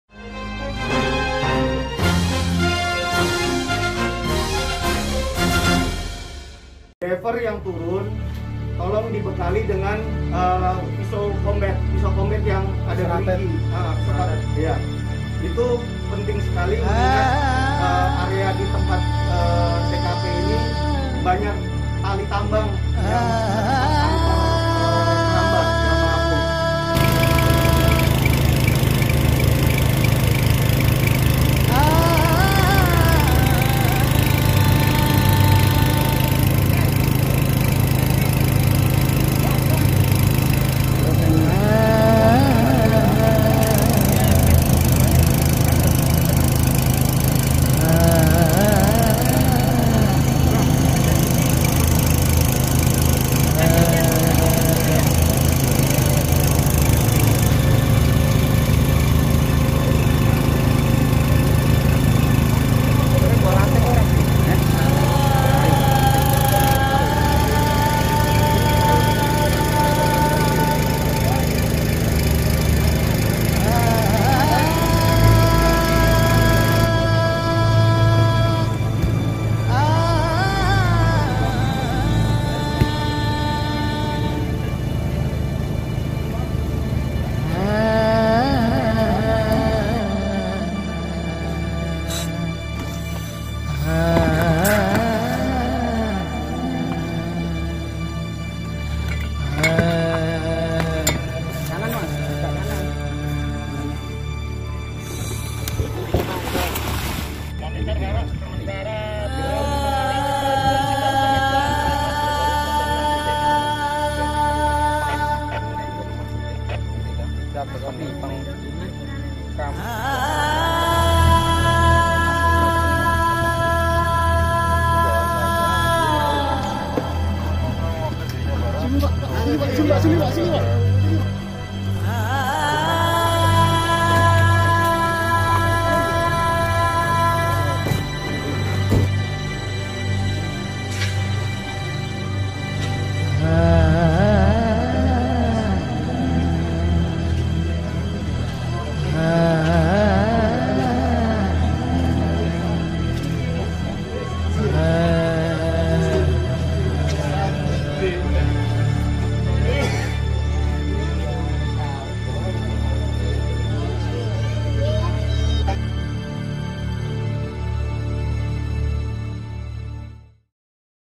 DIDUGA KELEBIHAN MUATAN PERAHU WISATA TERBALIK
Perahu wisata sarat penumpang, tenggelam di perairan Waduk Kedungombo, di wilayah Bulu, Desa Wonorejo, Kecamatan Kemusu, Boyolali, Sabtu pagi pukul 10.00. Dari 20 penumpang yang ada di atas perahu, 11 orang bisa diselamatkan warga setempat. Pencarian SAR telah menemukan enam orang dalam keadaan tewas, tiga lainnya masih dalam pencarian.

Koordinator Pos Badan SAR Nasional Surakarta, Arief Sugiyarto kepada Timlo.net menyebutkan pihaknya bersama sejumlah relawan SAR telah membentuk tim gabungan untuk menemukan para korban. Saat itu, saksi menyebut menjelang perahu merapat di Warung Apung, sejumlah penumpang ingin berfoto di bagian haluan perahu. Akibatnya, perahu tidak imbang. Air masuk melalui bagian depan dan akhirnya membuat perahu terbalik,” ujar Arief.

Sejumlah warga di sekitar lokasi, langsung menolong dengan perahu dan berhasil menyelamatkan 11 penumpang. Namun, sembilan penumpang termasuk beberapa anak-anak sudah telanjur tenggelam. Kemungkinan, mereka tidak mengenakan pelampung yang tersedia di perahu. Kepala Kantor Basarnas Semarang, Nur Yahya dalam siaran pers Sabtu malam menyebut, sembilan korban adalah Siti Mukaromah, Wilda, Niken Safitri, Tituk Mulyani, Ana, Zamzam, Jalal, Jalil dan Desti.